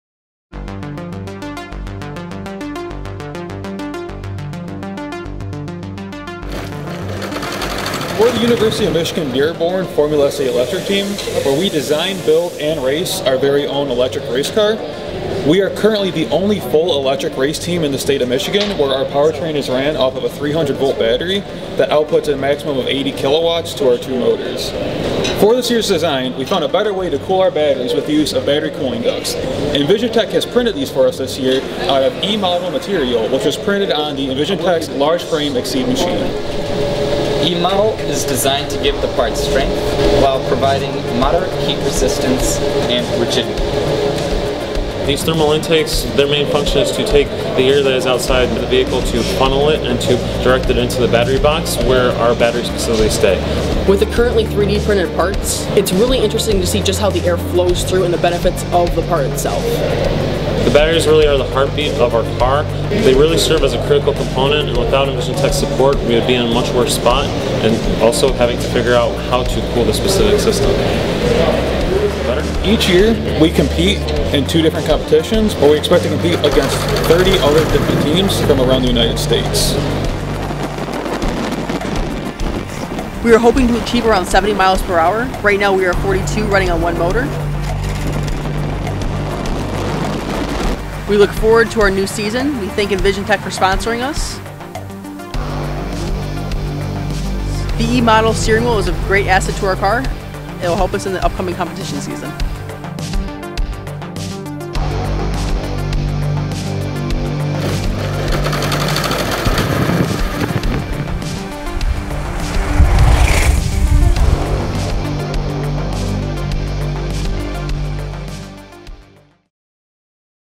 3D Printed Parts for Electric Race Car
EnvisionTEC has partnered with the College of Engineering and Computer Science student organization, Formula SAE Electric Racing Team at the University of Michigan-Dearborn to provide 3D printed battery cooling ducts and steering wheel for their electric race car.

The battery is the heartbeat of the electric car. With the 3D printed parts, students can create a steady flow of cold air / coolant to the battery pack of the electric vehicle in order to maintain low temperatures and improve battery life.
For effective cooling of these batteries, the cooling ducts were printed in E-Model 3SP on EnvisionTEC’s Xede 3SP 3D printer.

Learn more about EnvisionTEC’s 3SP technology for 3D printing in a large build envelope

For more information on the Xede 3SP 3D printer or E-Model 3SP 3D print material, visit: